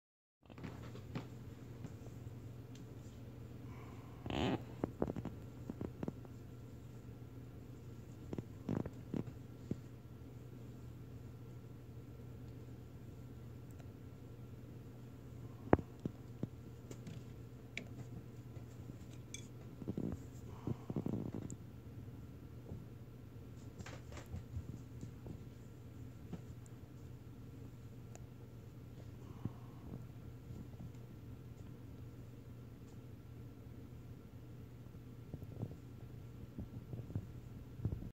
Gila monster eating an egg yolk
This is my gilas first time eating a raw egg, seems to be enjoying it! I broke the yolk for her her because she could not get a bite out if it.